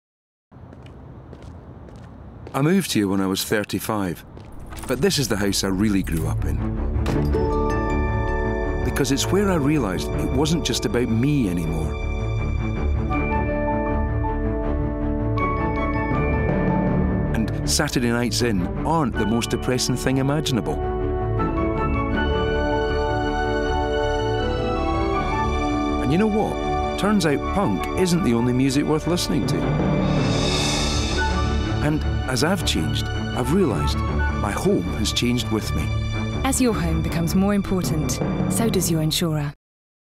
The House I Grew up in - New Home Insurance TV advert | Hiscox
The ‘house I grew up in’ uses striking projection mapping technology to tell the story of how your home becomes more important as you progress through life.